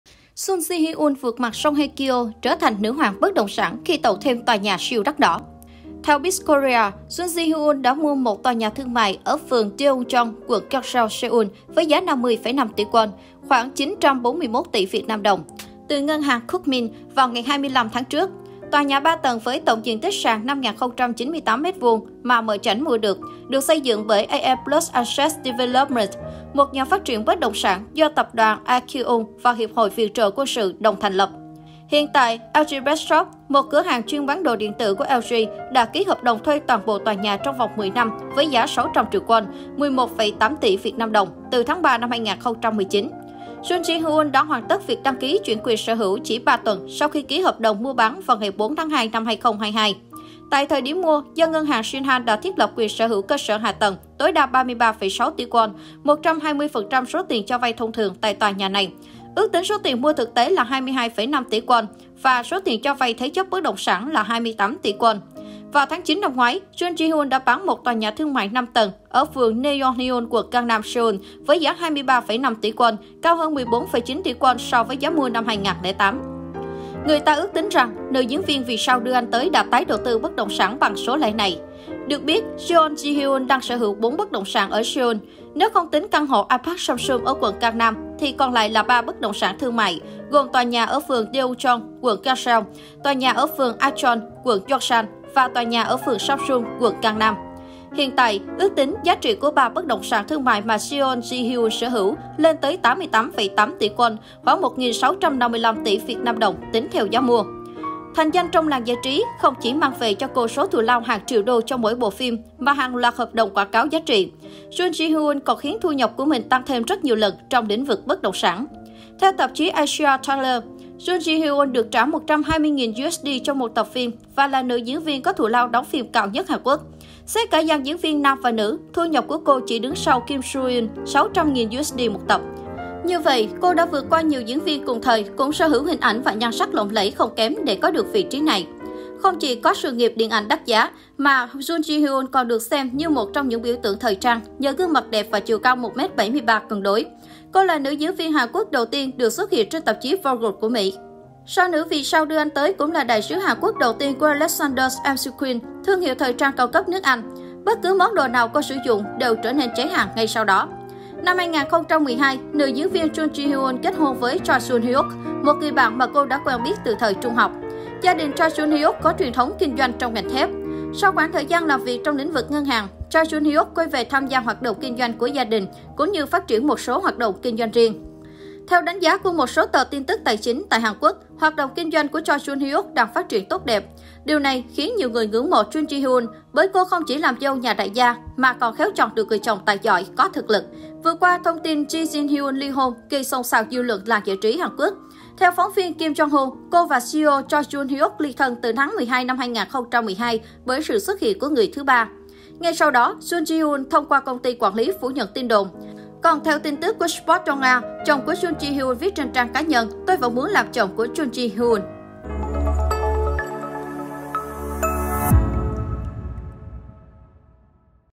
Jun Ji Hyun vượt mặt Song Hye Kyo, trở thành nữ hoàng bất động sản khi tậu thêm toà nhà siêu đắt đỏ
Theo Biz Korea, Jun Ji Hyun đã mua một tòa nhà thương mại ở phường Deungchon, quận Gangseo, Seoul với giá 50,5 tỷ won (khoảng 941 tỷ VNĐ), từ Ngân hàng Kookmin vào ngày 25 tháng trước. Tòa nhà 3 tầng với tổng diện tích sàn 5098 m² mà “mợ chảnh” mua, được xây dựng bởi AM Plus Asset Development - một nhà phát triển bất động sản do Tập đoàn Aekyung và Hiệp hội viện trợ quân sự đồng thành lập.